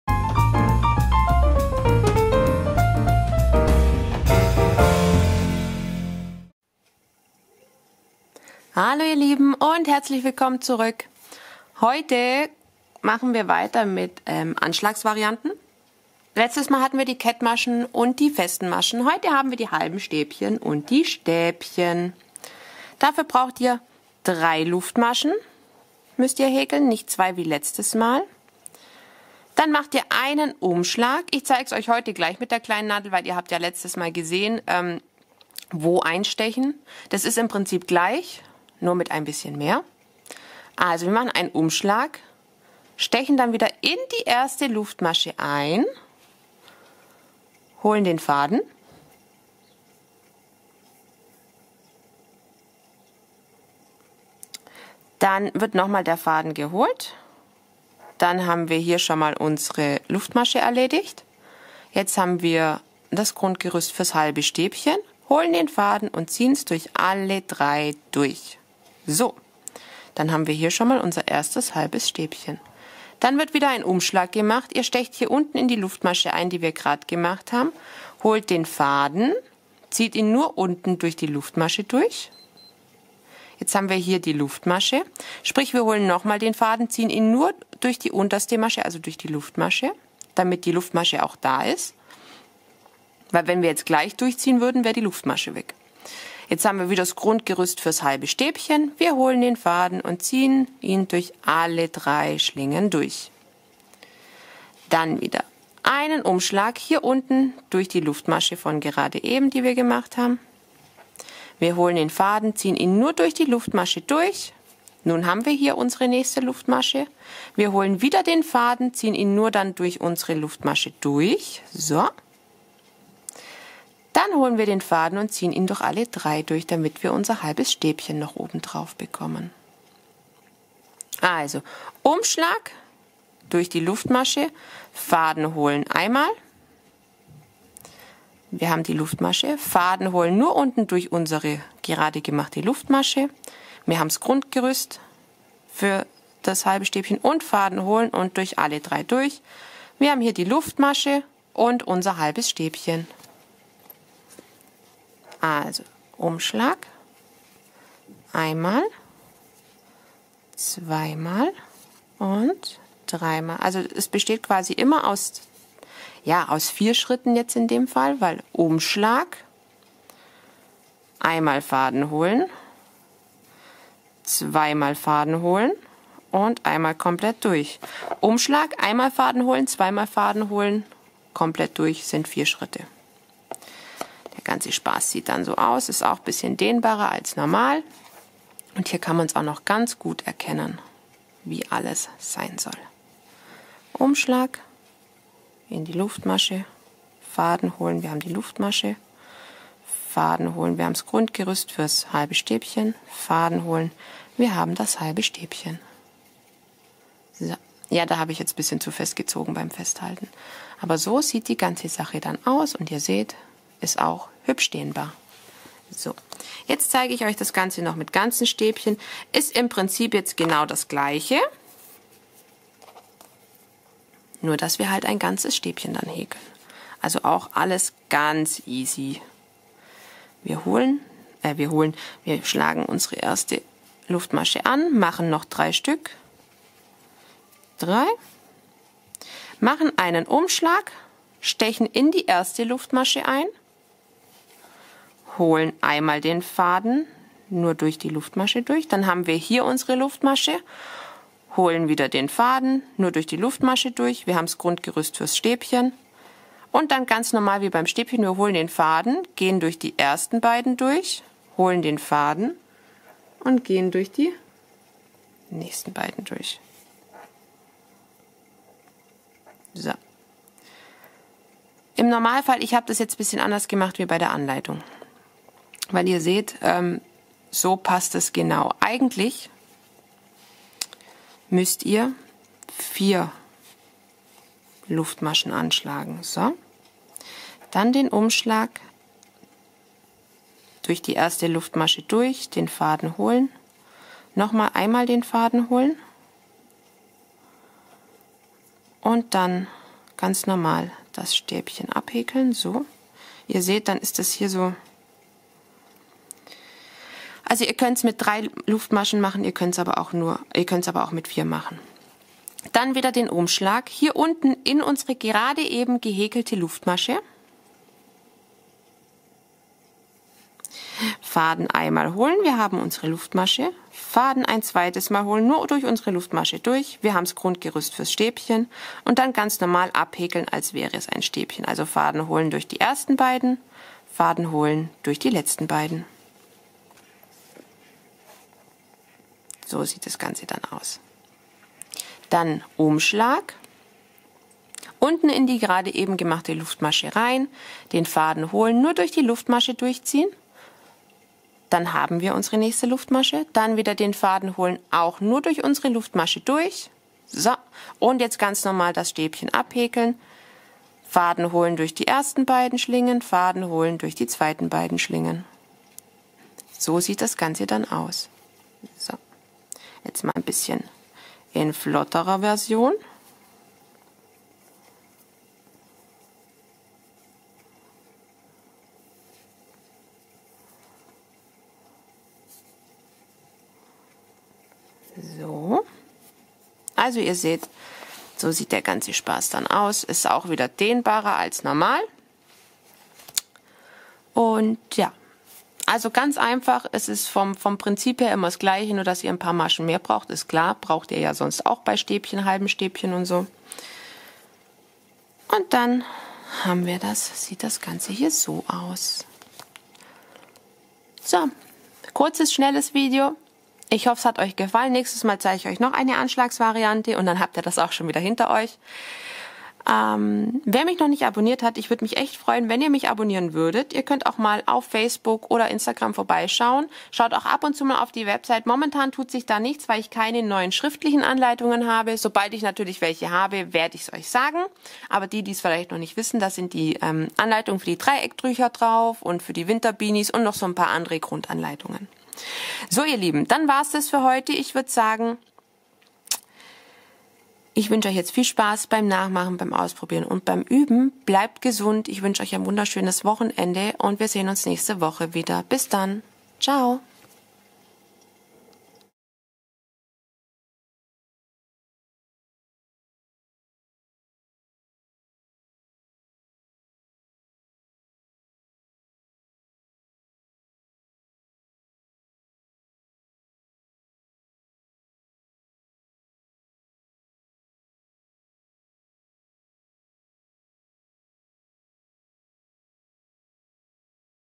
Maschenanschlag mit halben und ganzen Stäbchen häkeln
Heute zeige ich euch, wie man einen Anschlag aus halben und ganzen Stäbchen macht. Ich hoffe, es gefällt euch und wünsche euch viel Spaß beim Nachmachen, Ausprobieren und Üben und ein wunderschönes Wochenende.
Bleibt gesund!
Liebe Grüße,
eure Alex